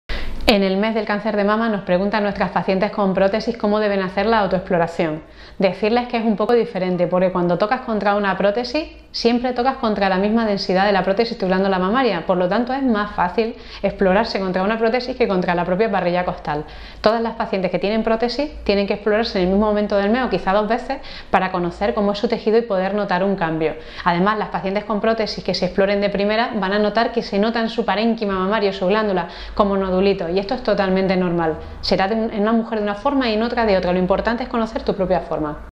Autoexploración mamaria: ¿es diferente si llevo implantes mamarios?
Llegó octubre, y con él nos vestimos de rosa como cada año. El día 19 de octubre se celebra el día Mundial de la lucha contra el Cáncer de Mama. Todos los años dedicamos nuestro blog a dar información de interés relacionada con la prevención del cáncer de mama, y este año no va a ser menos. Nos hemos querido centrar en una pregunta muy típica entre nuestras pacientes de aumento de pecho, y es la siguiente: “¿Cómo se hace la autoexploración mamaria si llevo prótesis?, ¿es exactamente igual?, ¿se hace de forma diferente?”. Hoy, la Dra. Martínez Padilla os contamos todo lo relacionado con la autoexploración mamaria con implantes mamarios